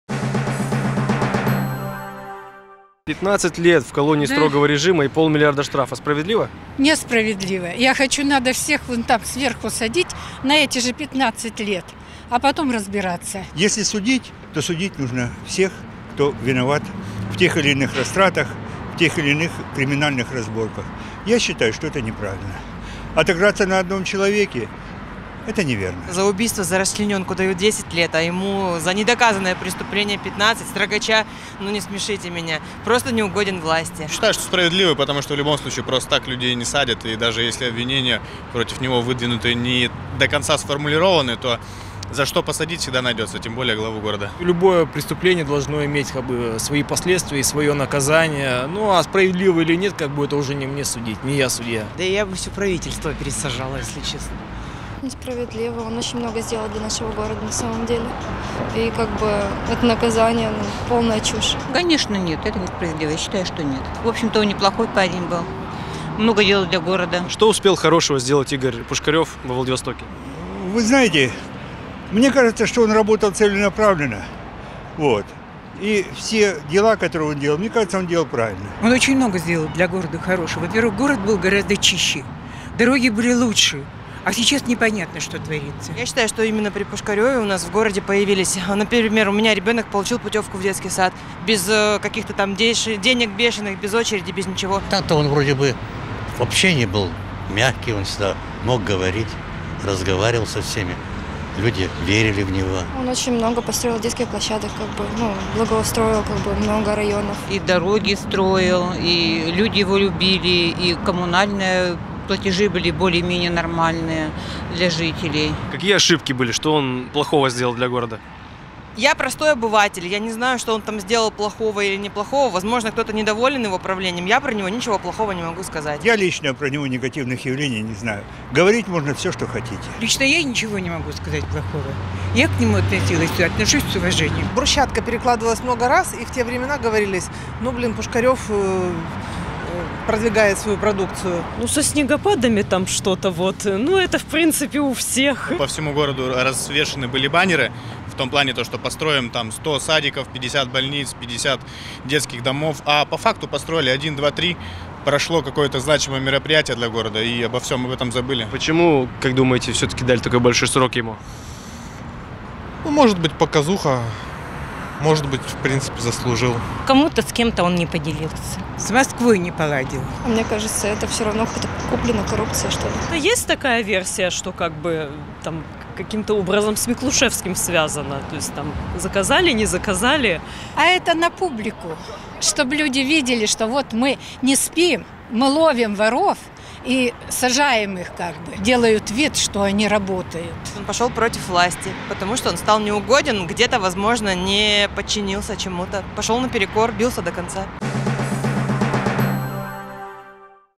VL.ru - Что горожане думают о наказании Игоря Пушкарева (ВИДЕОБЛИЦ)
Во вторник, 9 апреля, экс-мэра Владивостока Игоря Пушкарева приговорили к 15 годам лишения свободы в колонии строгого режима и штрафу в 500 миллионов рублей. VL.ru узнал у жителей столицы Приморья, считают ли они решение справедливым, за что, по их мнению, посадили бывшего градоначальника, а также о позитивных и негативных моментах восьми лет правления Пушкарева.